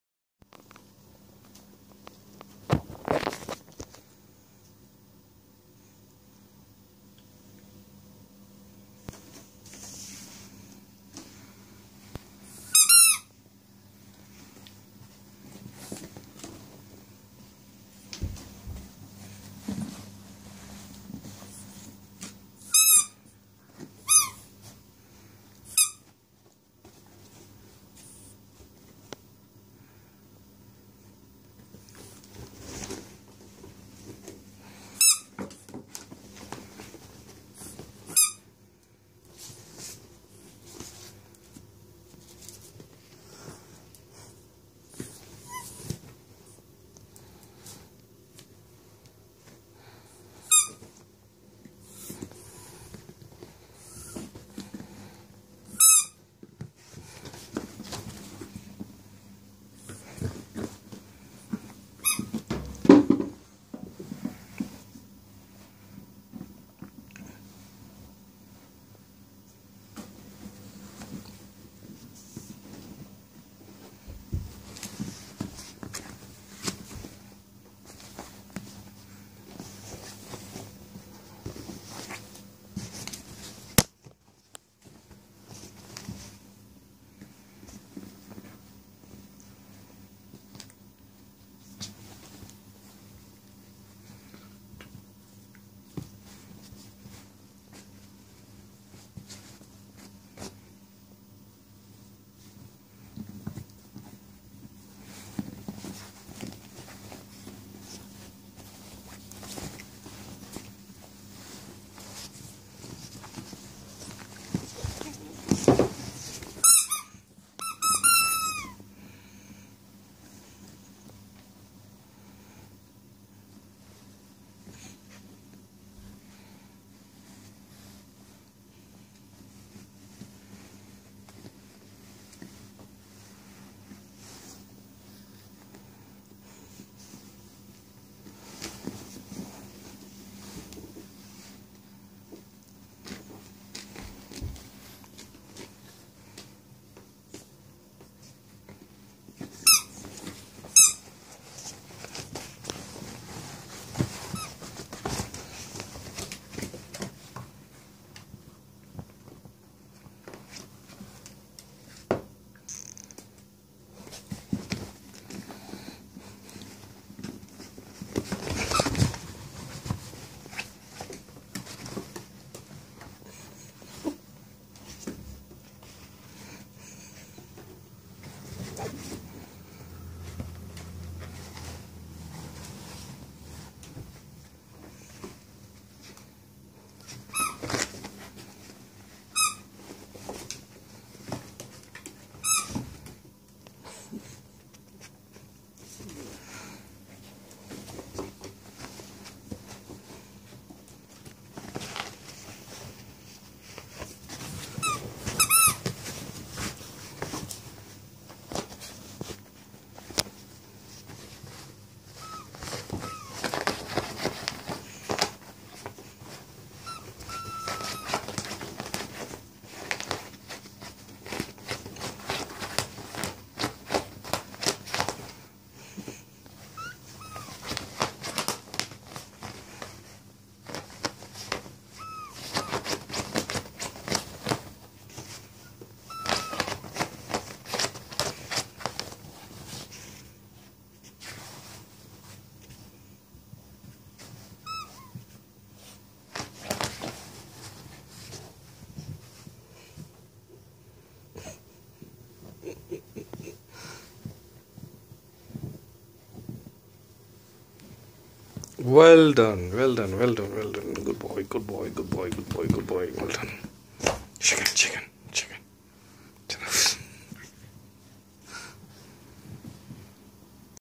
Yorkshire terrier ultimate performance
just have few minutes fun with our cookie boy.